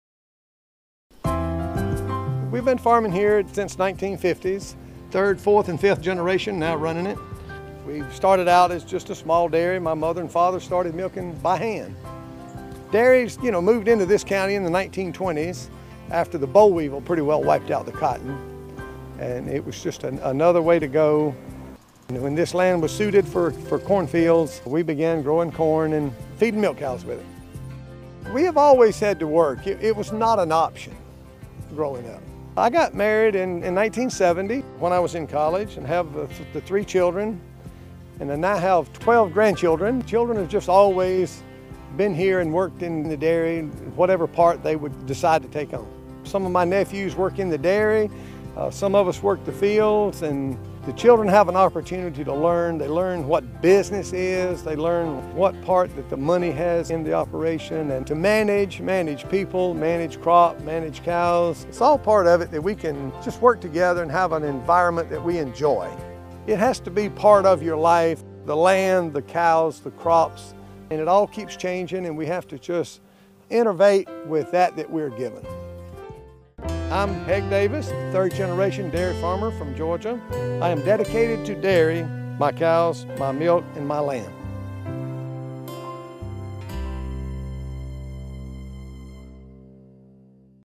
Meet the Davis Dairy Farm Family of Georgia
Georgia dairy farmer Heck Davis knows that family is the key to success. Together, with his brothers, Russell and Jack, they operate Green Glades Farm in Eatonton, Ga. The Davis brothers milk about 300 cows on a 450-acre farm that is also used to grow sweet corn and forage crops. Davis said that with a changing marketplace that is more global than it is local, the brothers have worked to diversify their operations. Davis takes great pride in how his family has worked together and built successful lives. “My brothers and I, we are more than a family, we are a team,” he said.